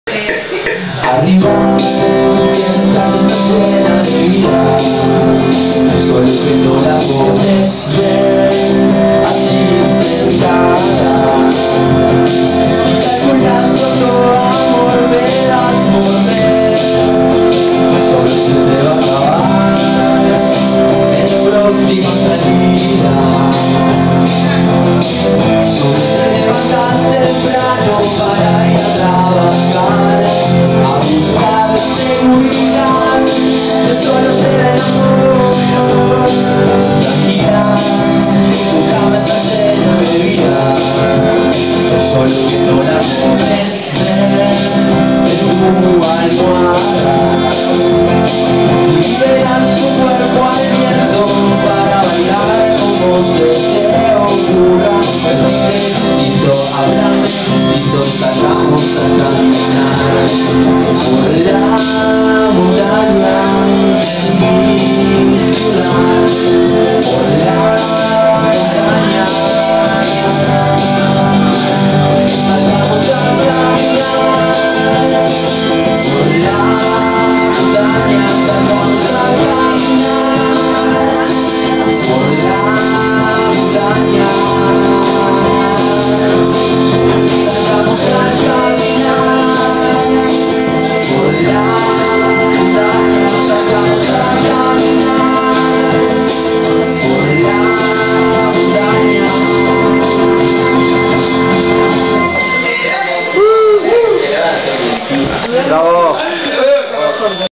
Federico Salas en vivo en Prana pelu IV villa urquiza, 2014
Ciclos en Prana por Law Pop
30 de enero 2014
Presentan:
Federico Salas en vivo
Video x LaW PoP
Y la BueNa =)nDa De LoS ChicOs De PraNa!!
Un ciclo para ReFrEsCaRtE!